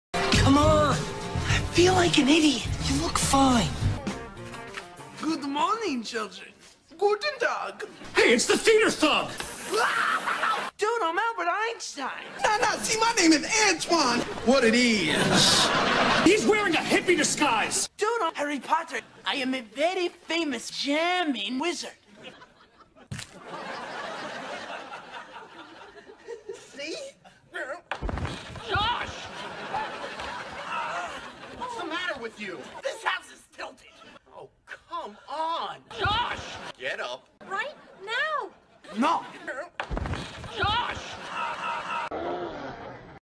YTP - Josh's Identity Crisis
Josh proves he's a wizard by falling down.